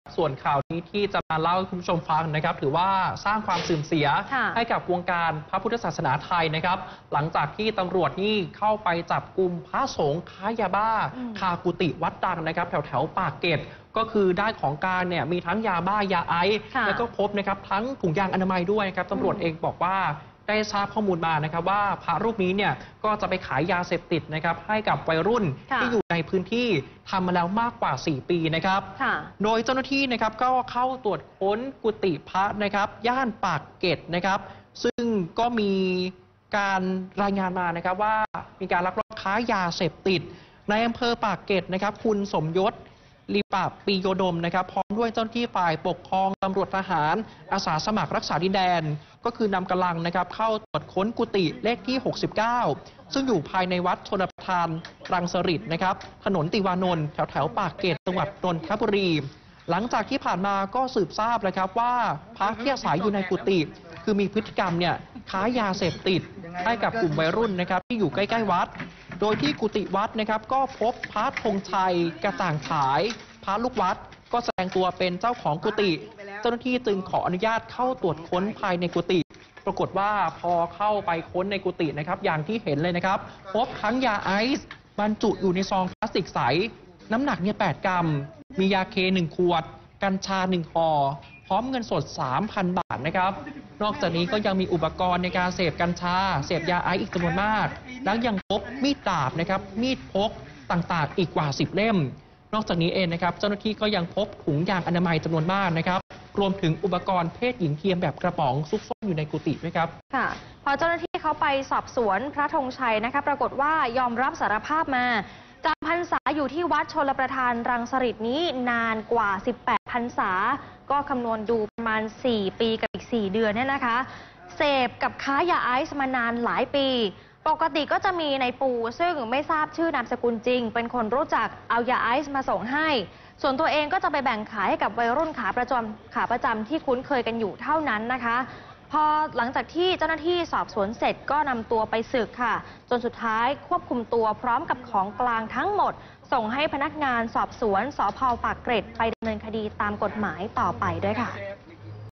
News รวบพระค้ายาไอซ์คากุฏิ
พิกัดข่าวเช้า : รวบพระค้ายาไอซ์คากุฏิ สารภาพแบ่งขายวัยรุ่นในพื้นที่นานหลายปี "NOW26" 30-6-59